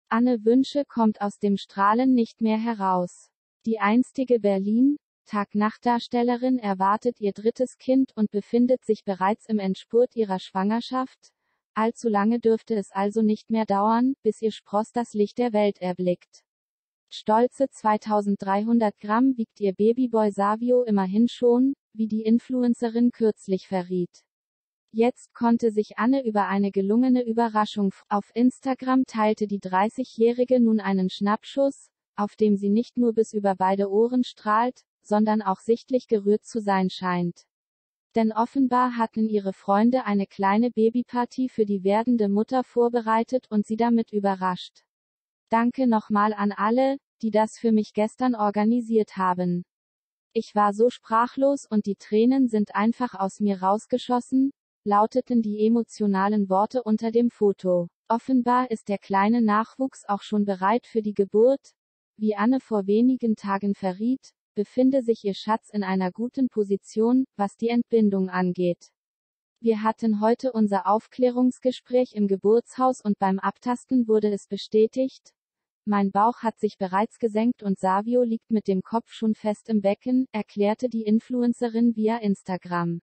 Emotionaler Moment: Anne Wünsche mit Babyparty überrascht
Anne Wünsche (30) kommt aus dem Strahlen nicht mehr heraus! Die einstige Berlin - Tag & Nacht-Darstellerin erwartet ihr drittes Kind und befindet sich bereits im Endspurt ihrer Schwangerschaft – allzu lange dürfte es also nicht mehr dauern, bis ihr Spross das Licht der Welt erblickt. Stolze 2.300 Gramm wiegt ihr Babyboy Sávio immerhin schon, wie die Influencerin kürzlich verriet. Jetzt konnte sich Anne über eine gelungene Überraschung freuen!